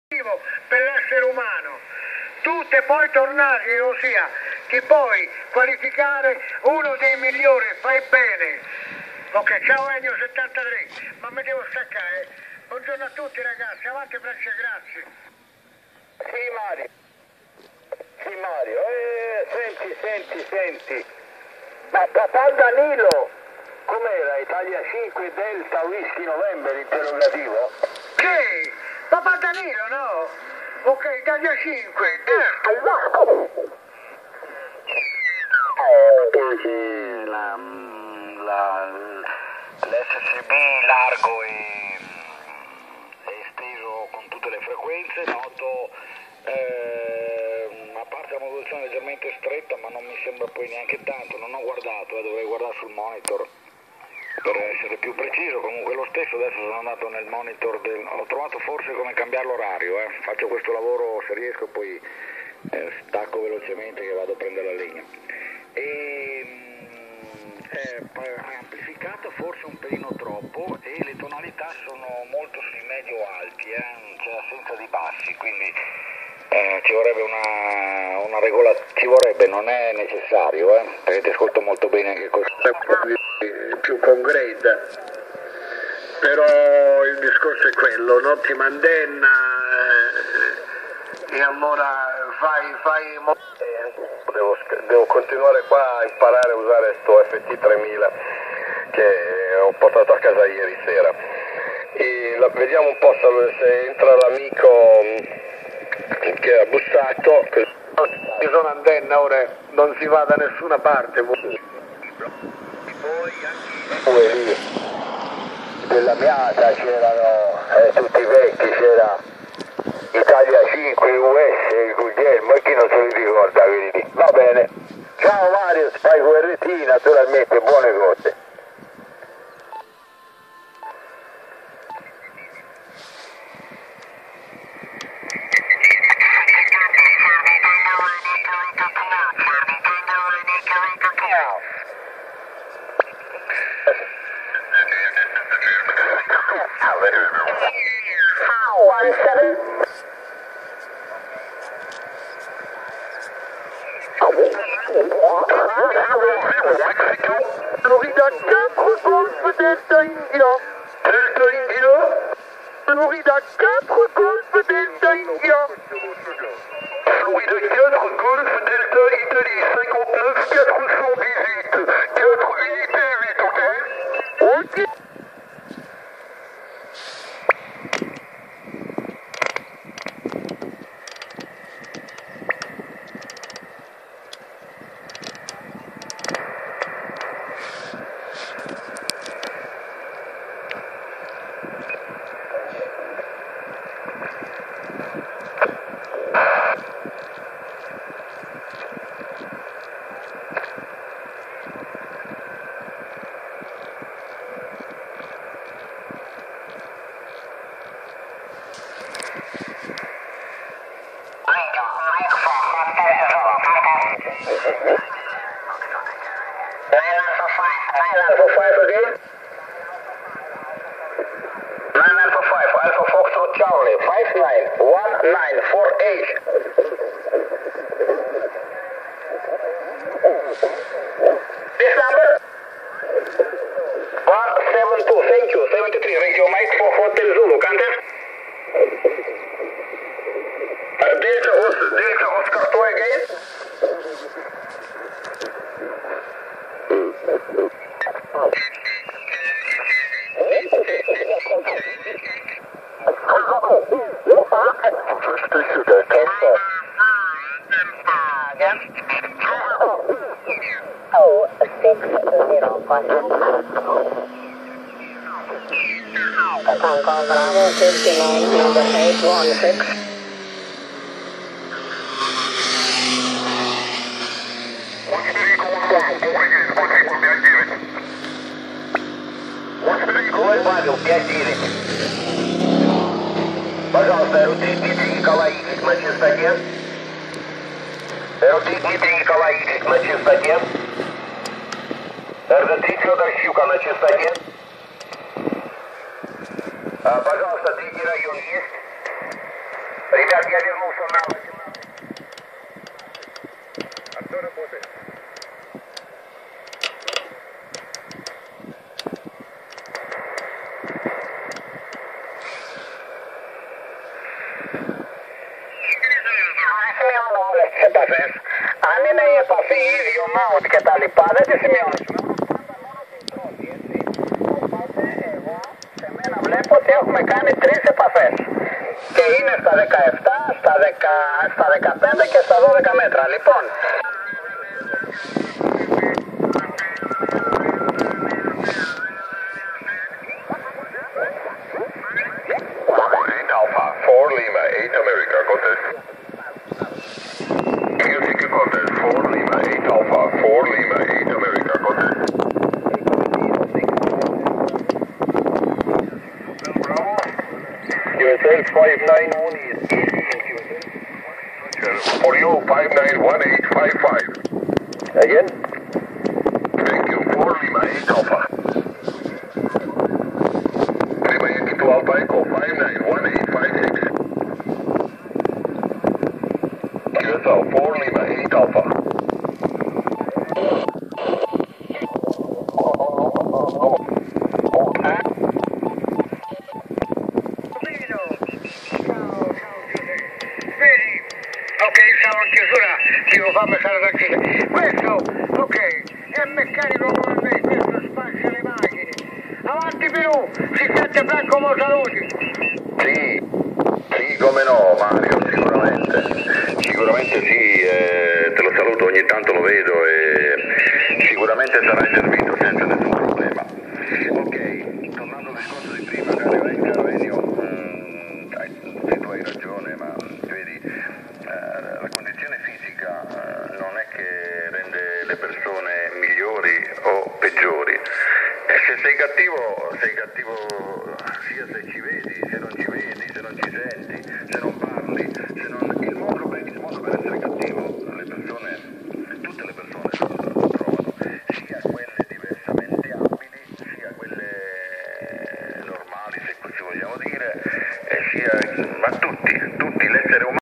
Prove ricezione FT 817 Monte Majella con antenna made by Luca IZ8HLE
Mattinata trascorsa in montagna sul monte majella a circa 2000 metri di quota dove tra le altre cose ho avuto modo di provare in ricezione l'antenna ad uso portatile "stile" Miracle realizzata da Luca IZ8HLE. Purtroppo ho dimenticato il microfono ed il tasto a casa !!!